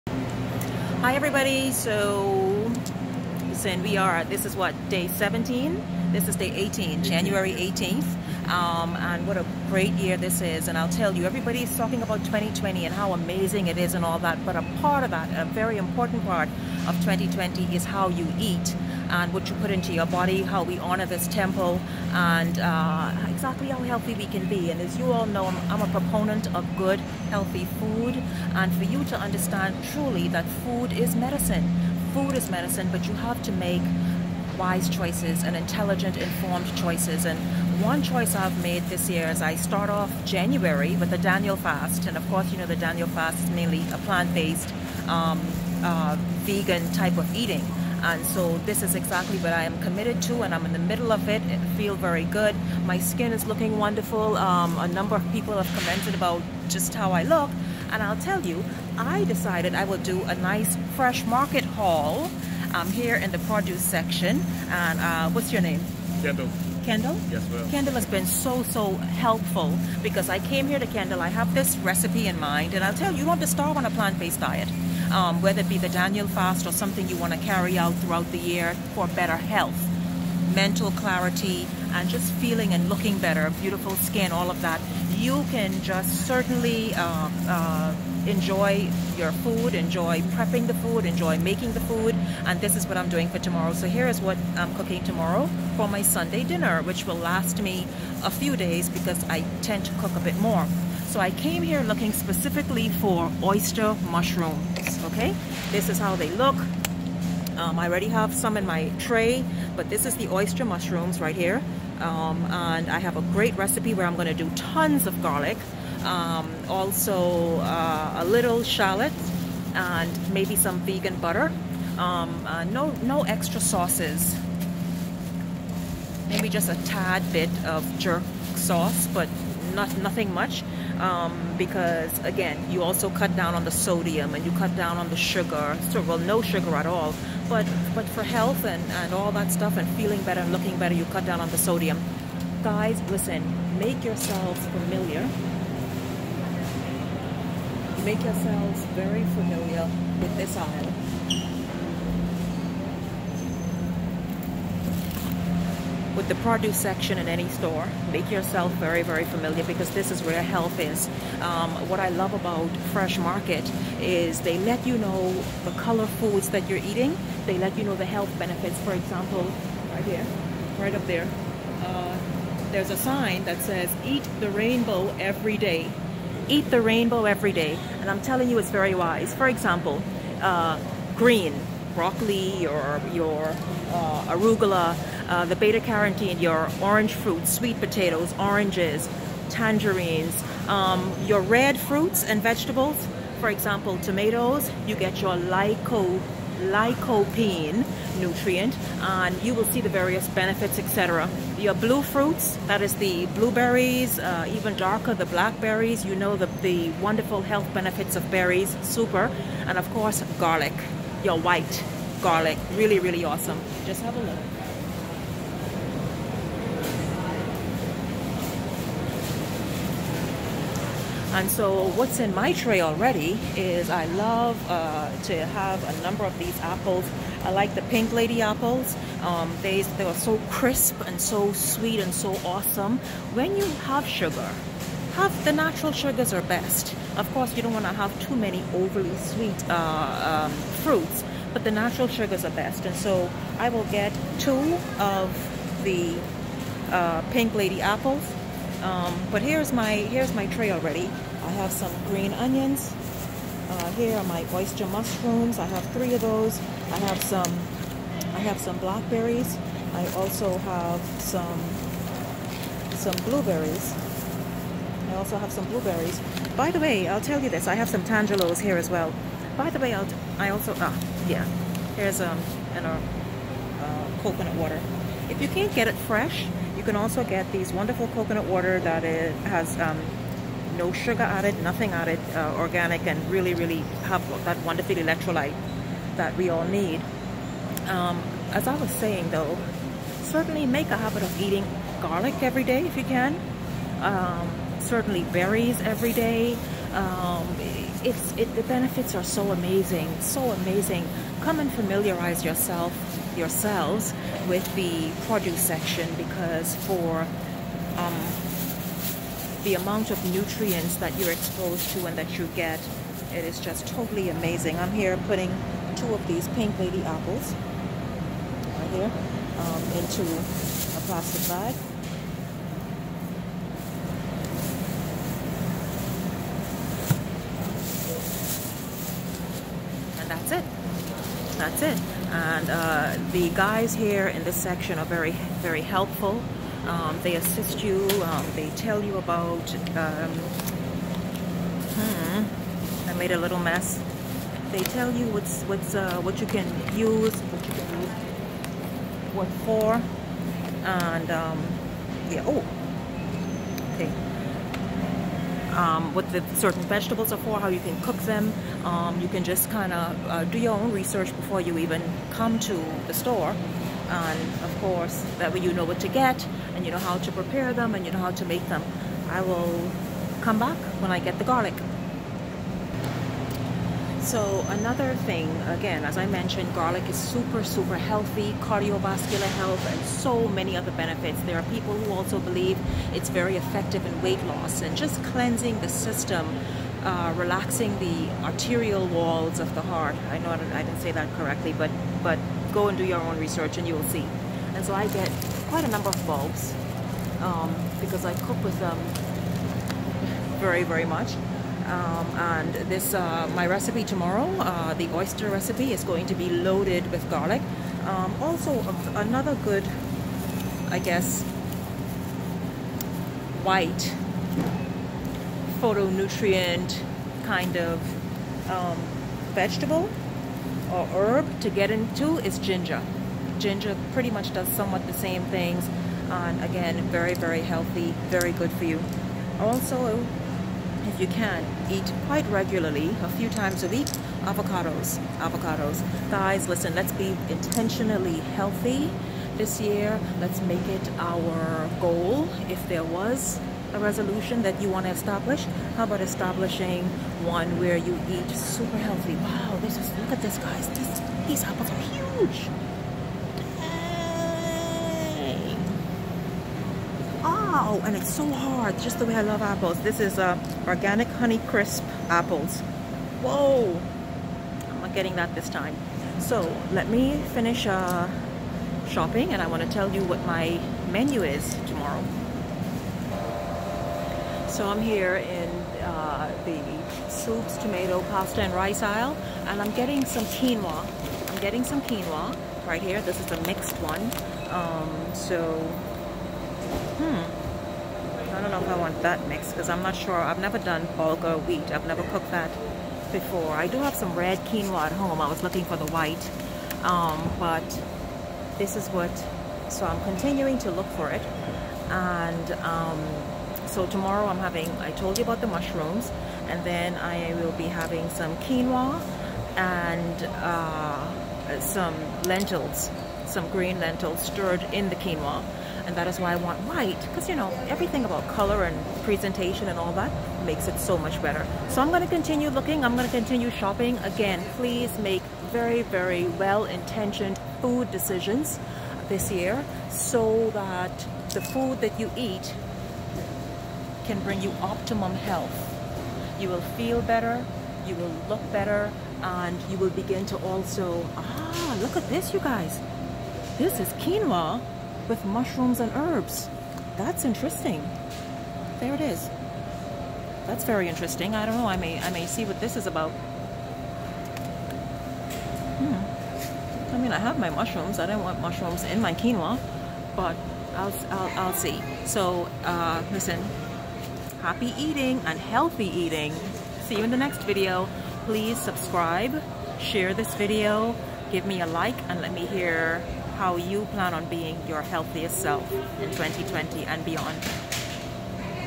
Grocery haul|| Fresh Market Old Fort Bay|| Recipe
I share a Sunday plant based recipe and I walk you through healthy options at the grocery store. Health is Wealth!